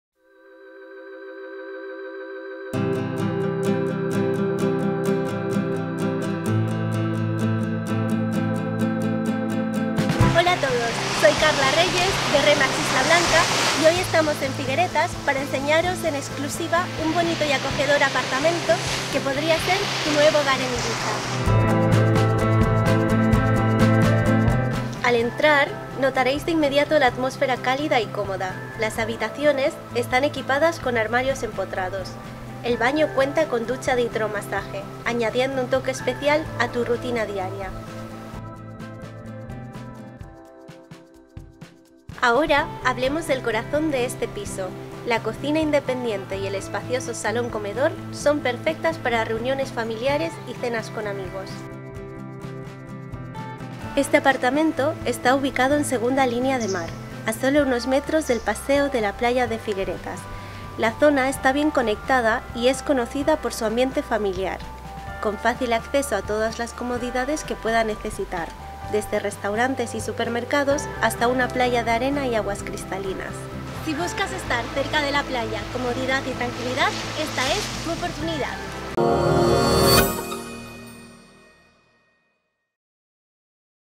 Bonito y acogedor apartamento en segunda línea de mar en Figueretas-Ibiza
Bienvenido a este cálido apartamento de 2 habitaciones con luz natural que fluye a través de sus ventanas y te brinda un ambiente cómodo y acogedor. Las habitaciones cuentan con armarios empotrados para satisfacer tus necesidades de almacenamiento, y el baño ofrece una ducha de hidromasaje añadiendo un toque especial a tu rutina diaria.

La cocina independiente y el amplio salón comedor son el corazón de este hogar, ideales para reuniones familiares y cenas con amigos.

Situado en segunda línea de mar, estás a solo unos pasos del bonito paseo de la playa de Figueretes. Esta zona es conocida por su ambiente familiar y su acceso a todas las comodidades que puedas necesitar, desde restaurantes, supermercados, parques y playas de arena de agua cristalina.

Experimenta la auténtica atmósfera de Ibiza mientras haces de este apartamento tu nuevo hogar en este encantador rincón de la isla.

¡Contáctanos hoy para programar tu visita!